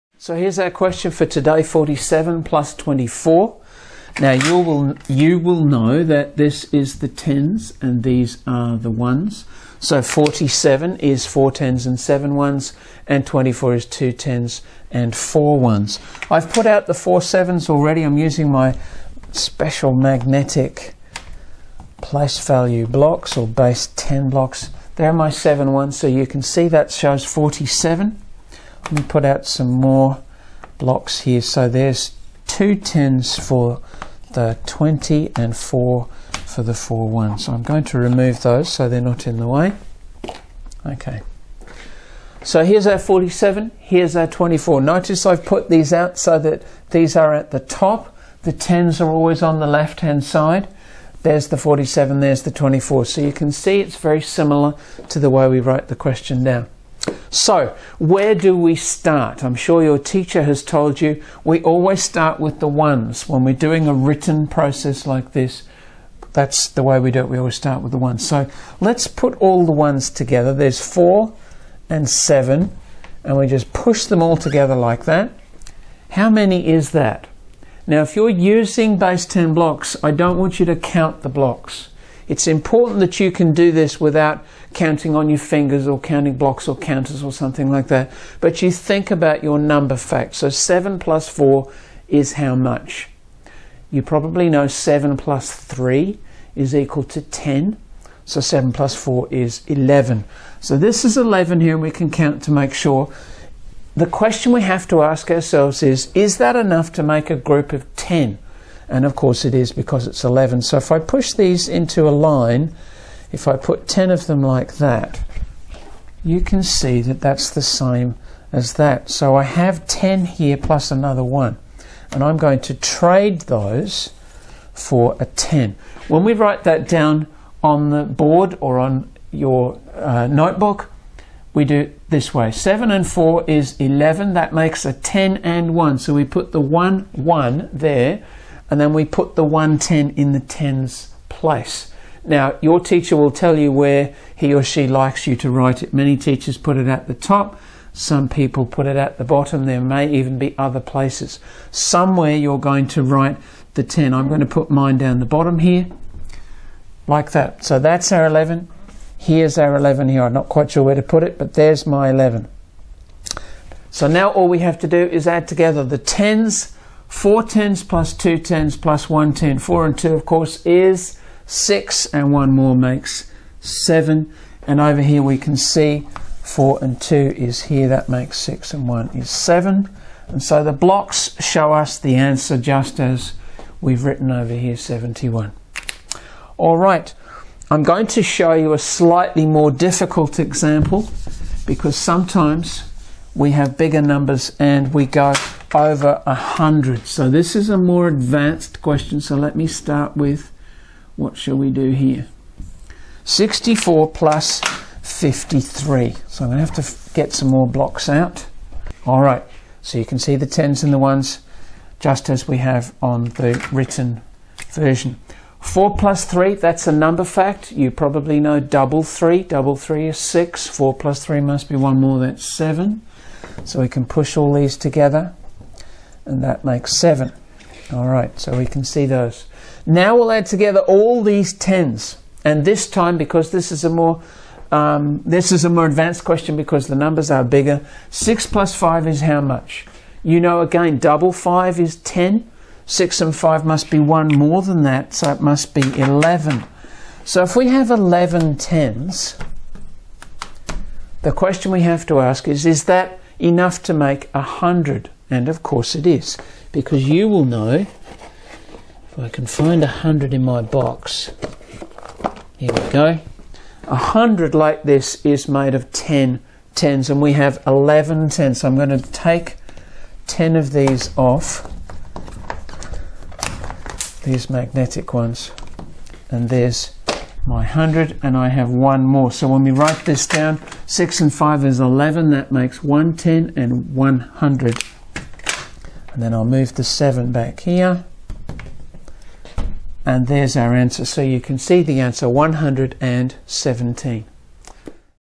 Learn the Addition Algorithm with Regrouping
Dr Peter Price explains the addition algorithm for students.

Students should learn to understand the algorithm as they practice it, by seeing each step modeled with base ten materials.